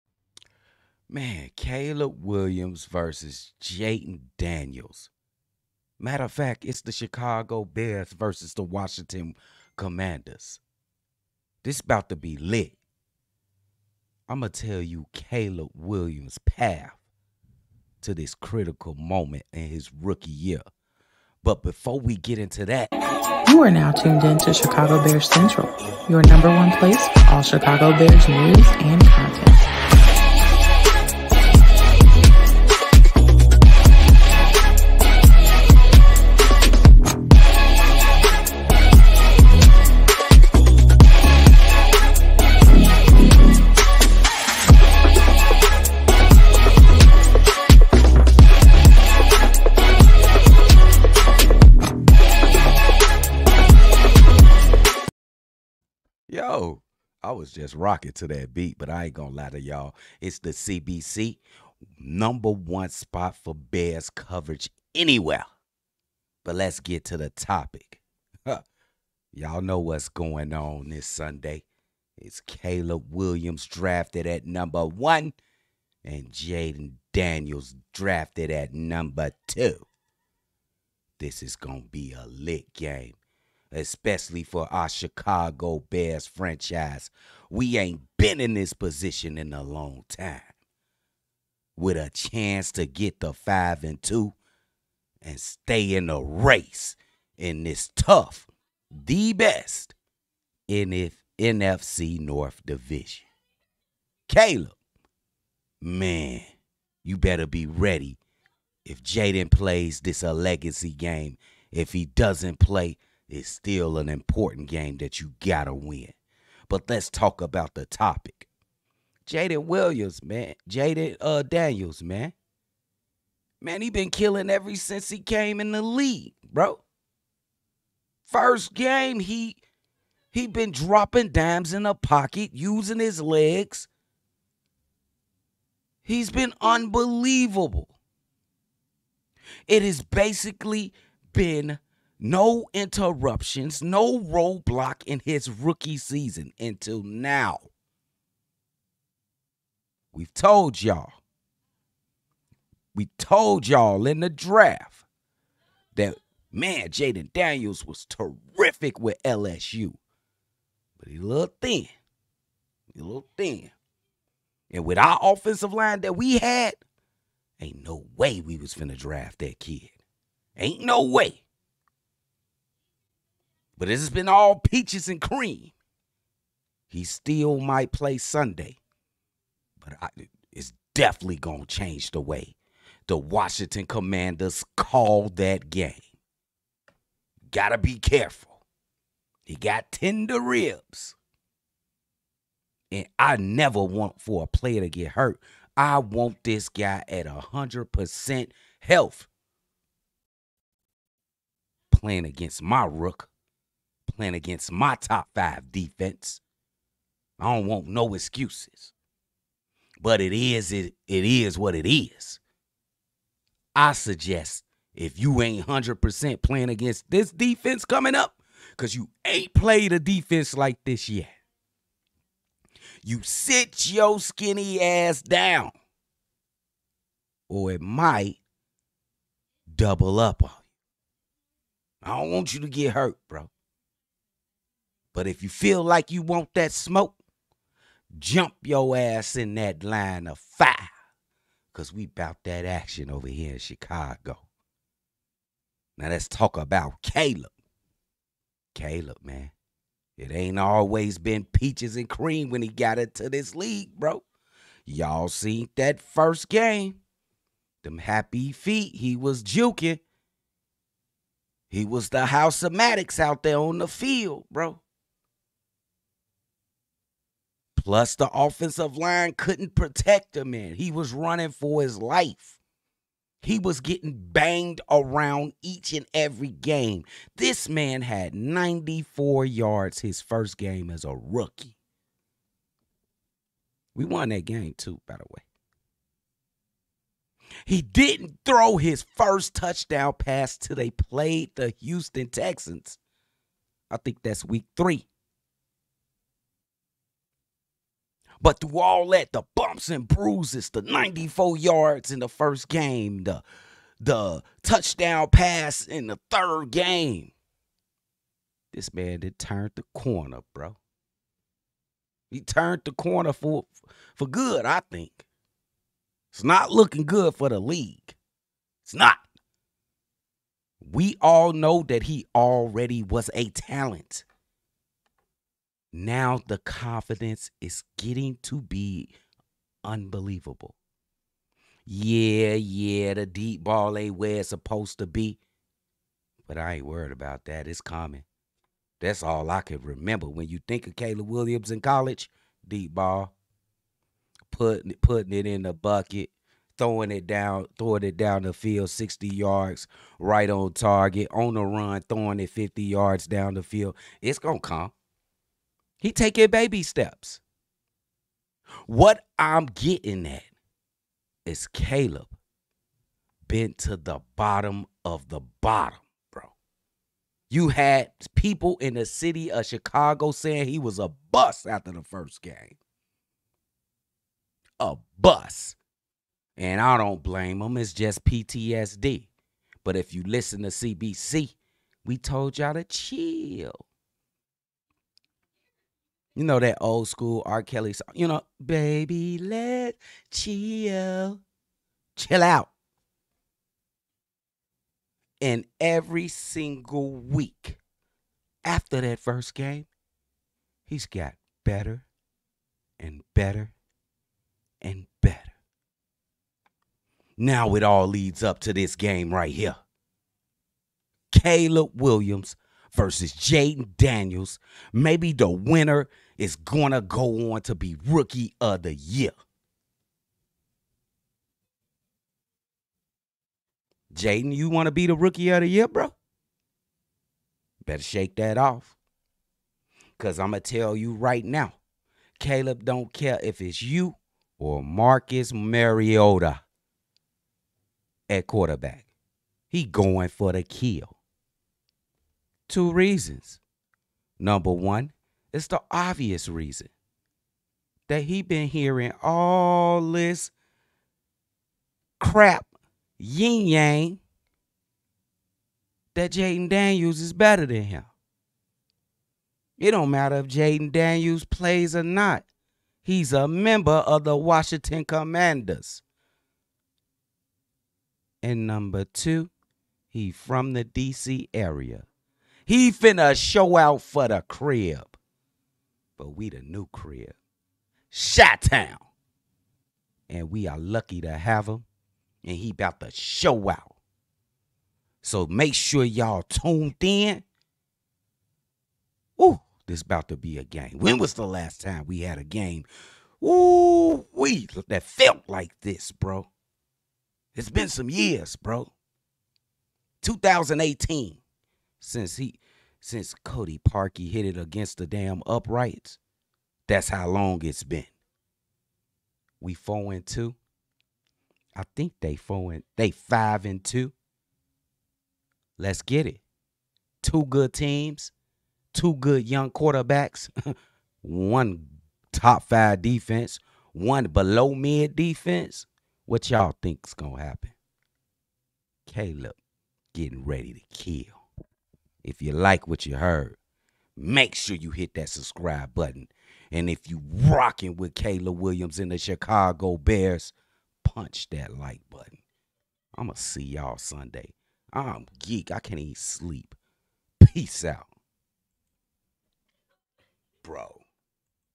Caleb Williams vs Jayden Daniels Hype!
Haize C-Dubb & Bobby discuss the upcoming matchup between Caleb Williams vs Jayden Daniels is sure to be a great one if Jayden shows up!

THE VOICE OF THE FANS!